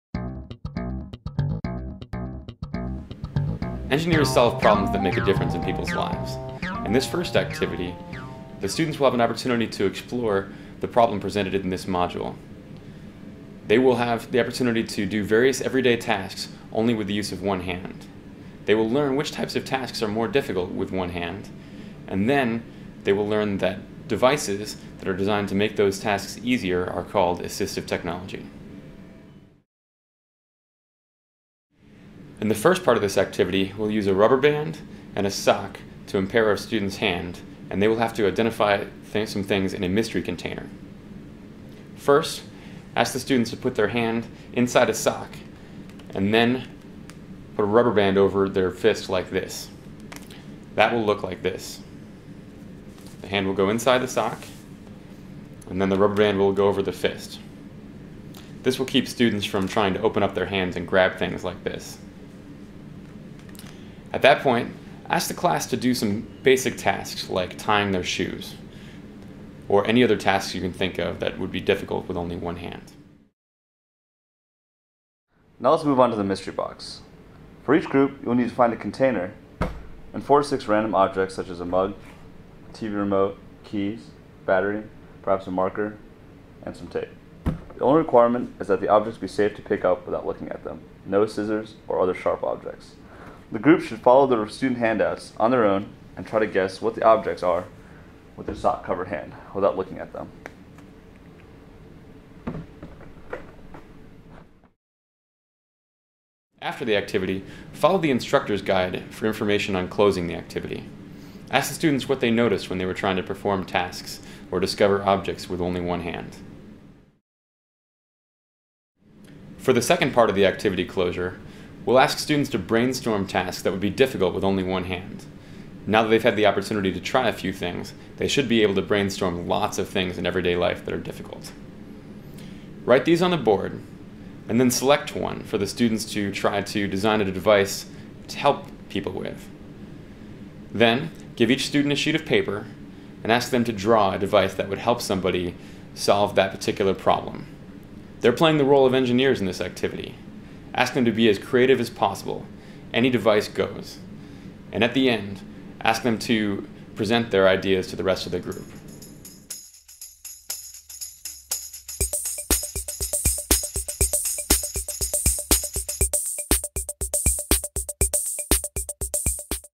Bionic Arm Activity 1
This video describes how to facilitate Activity 1 of the TechXcite: Discover Engineering Bionic Arm Module for middle school students. The playlist for this module includes videos to helping instructors facilitate all 5 activities in the module.

This TechXcite: Discover Engineering! module introduces kids to the ways in which engineers design technology to help people with disabilities.  They explore the design considerations of developing a prosthetic arm to improve the quality of life for someone who has lost their arm.  The module demonstrates ways in which design of assistive technology is interdisciplinary as the module combines mechanical, electrical, and biomedical engineering concepts. One activity allows students to explore hydraulic and pneumatic systems and teaches them about properties of gases and liquids. Another activity provides students the opportunity to apply hydraulic or pneumatic principles to make a mechanical arm move. The students create a rudimentary touch sensor showing them how electrical engineering may be utilized in the creation of a prosthetic. At the end of this module, each pair of students will have designed and built their own prosthetic arm.

The videos in this playlist provide you with the information you need to facilitate these activities with middle school students. Prior to facilitating the module, we recommend you build the projects yourself. Materials lists, instructor's guides, and youth handouts are all available on our website.